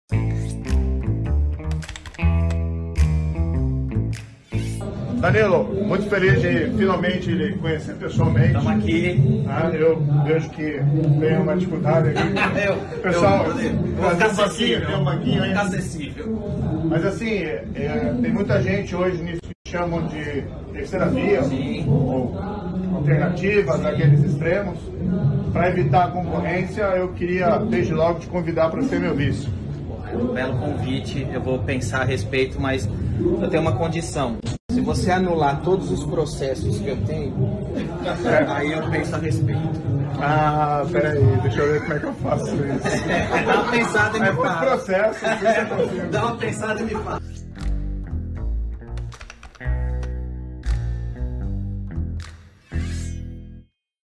SERGIO MORO CONVIDA DANILO GENTILI PARA SER SEU VICE NAS ELEIÇÕES 2022
Sergio Moro convidou, em tom de brincadeira, Danilo Gentili para ser seu vice. O ex-juiz da Lava Jato, que se filiou ao Podemos no último dia 10, e o apresentador participaram de evento do MBL, em São Paulo.

“Muita gente me chama de ‘Terceira Via’ […] e, para evitar a concorrência, eu queria te convidar para ser meu vice.”

Gentili respondeu:

“É um belo convite, eu vou pensar a respeito, mas eu tenho uma condição: se você anular todos os processos que eu tenho, aí eu penso a respeito.”